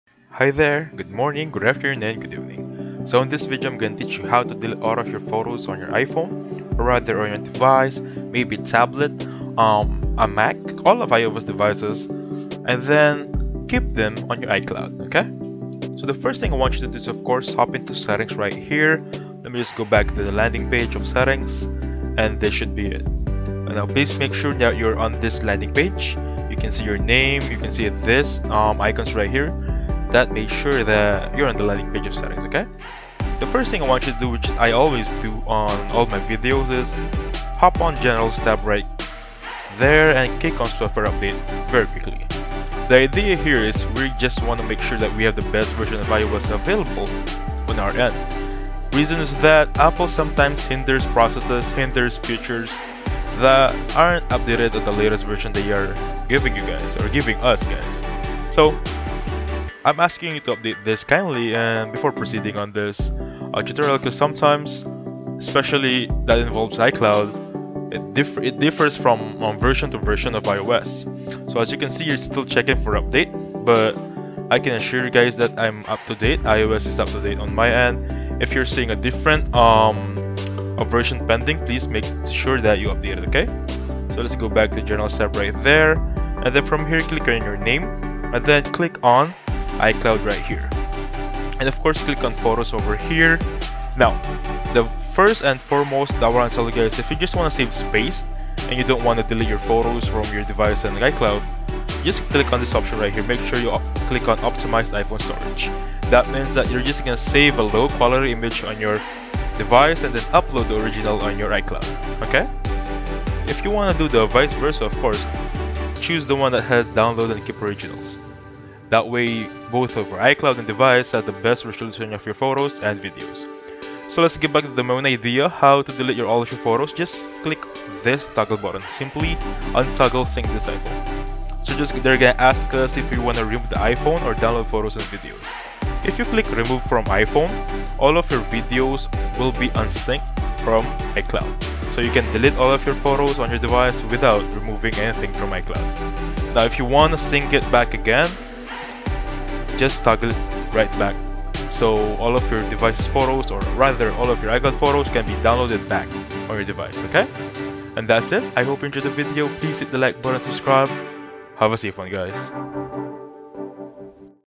How To Delete Photos Only On iPhone But Not iCloud
Need to free up space and manage your iCloud photos? Discover how to manage your iCloud Photo Library efficiently, delete photos from your iPhone without affecting your iCloud storage, and ensure your memories are in sync across devices.